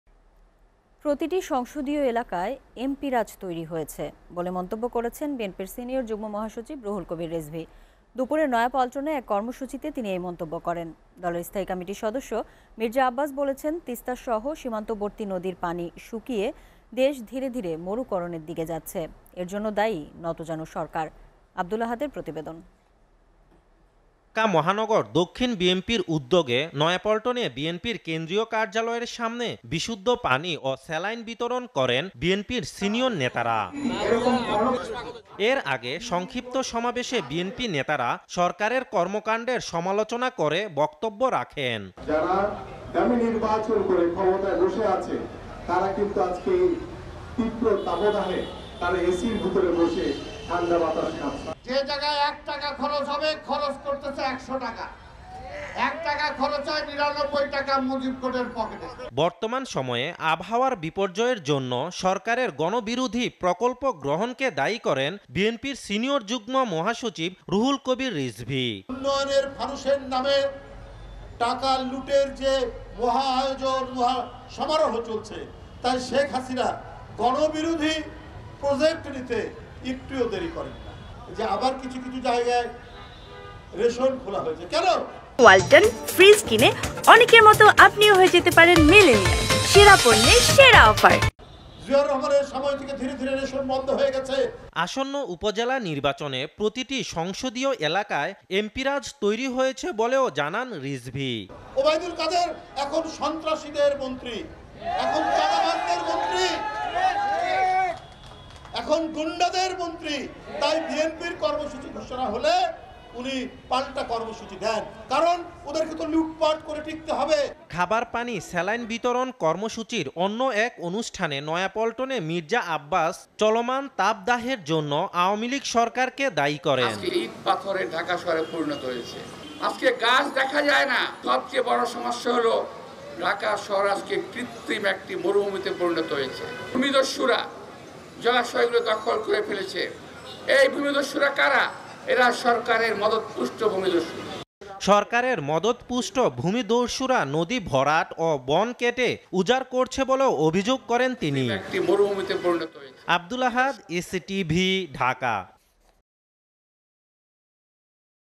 দেশজুড়ে 'এমপি-রাজ' সৃষ্টি করেছে ক্ষমতাসীনরা: রিজভী | BNP | SATV NEWS
দেশজুড়ে 'এমপি-রাজ' সৃষ্টি করেছে ক্ষমতাসীনরা: রিজভী

SATV Location:
Bangladesh